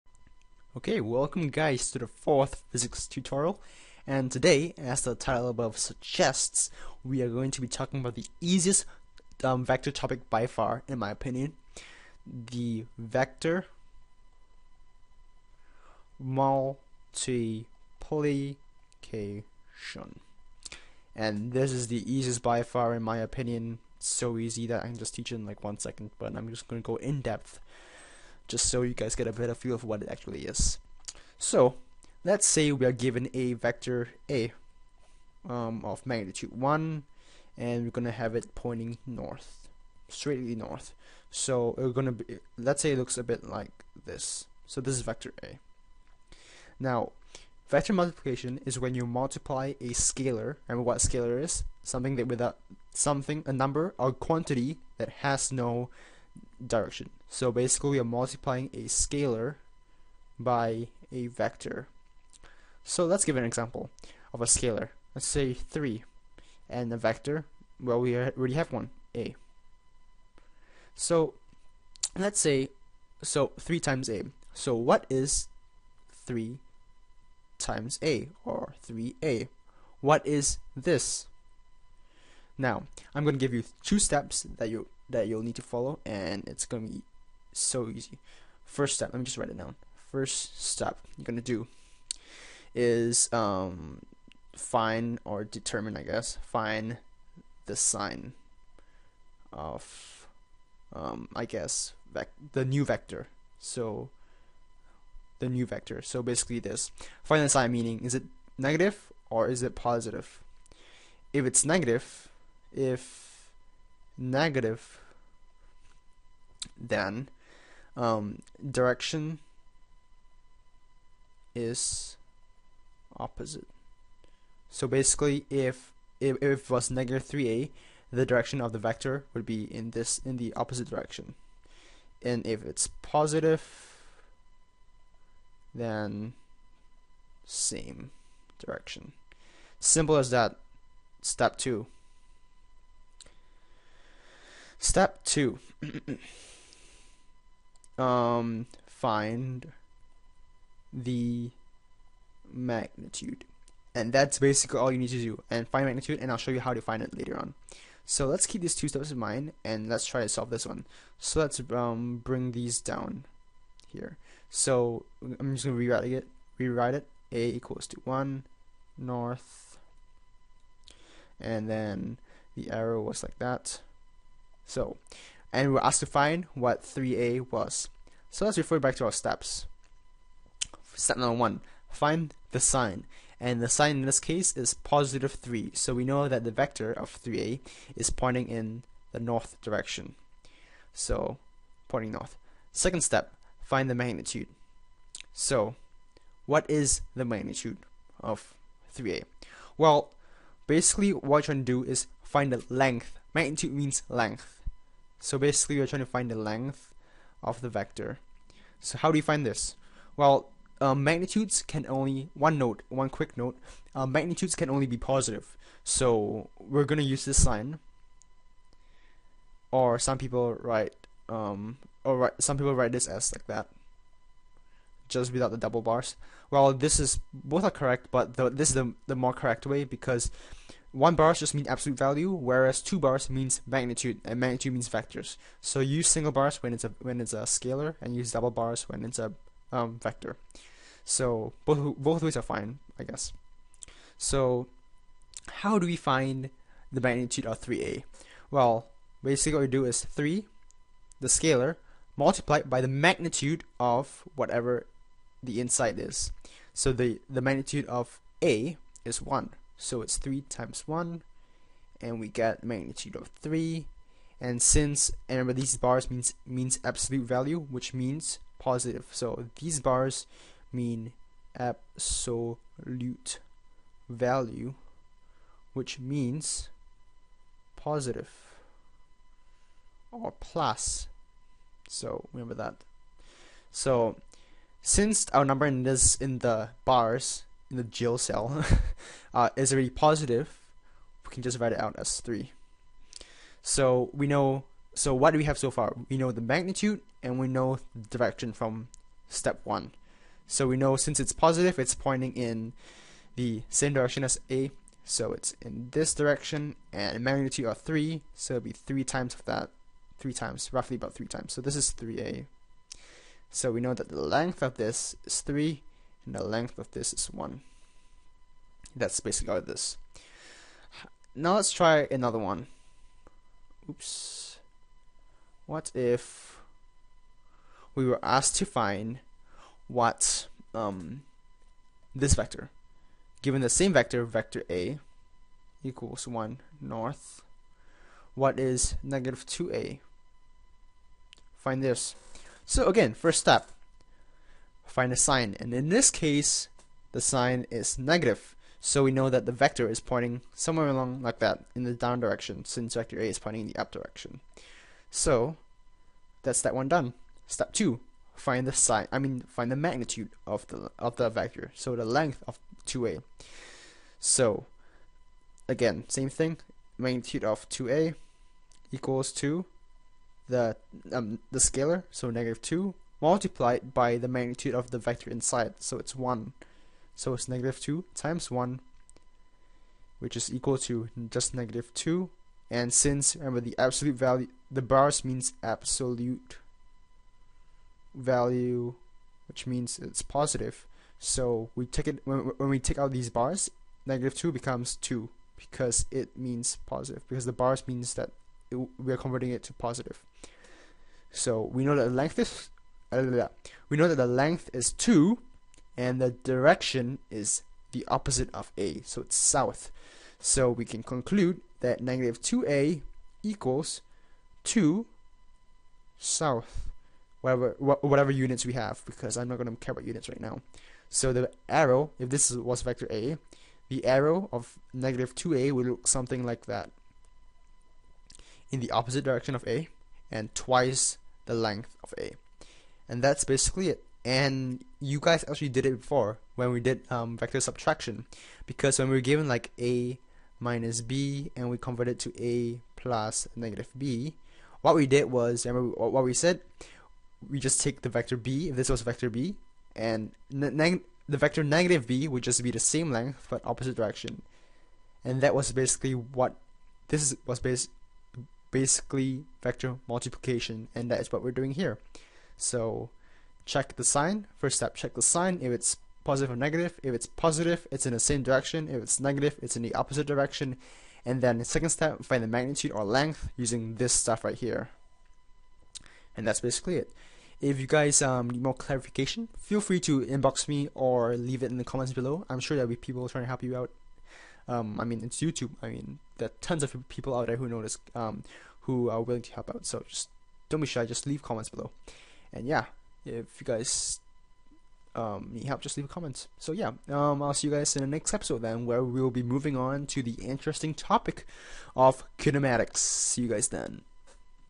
Vector Multiplication
This video will teach you how to multiply vectors by scalars.